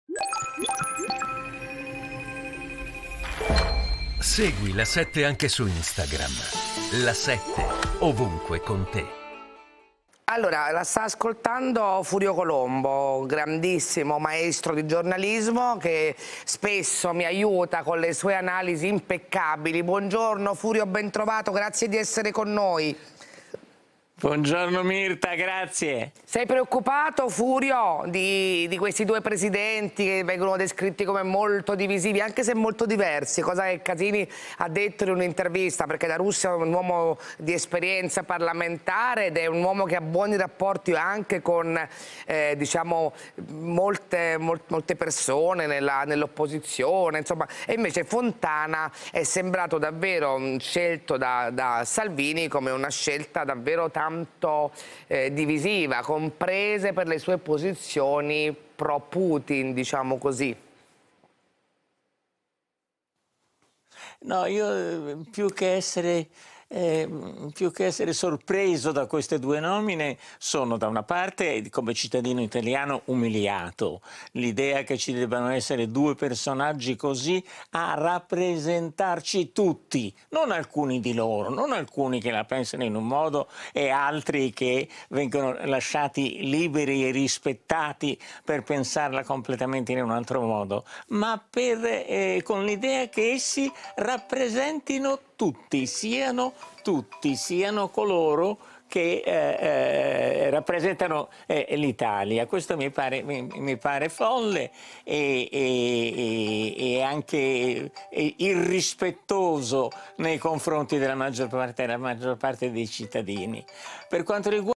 Furio Colombo sulle nomine di Ignazio La Russa e Lorenzo Fontana: "Sono umiliato come cittadino ...
Furio Colombo sulle nomine di Ignazio La Russa e Lorenzo Fontana alla presidenza delle Camere: "Due personaggi così a rappresentarci tutti, questo mi pare folle e irrispettoso nei confronti della maggior parte dei cittadini".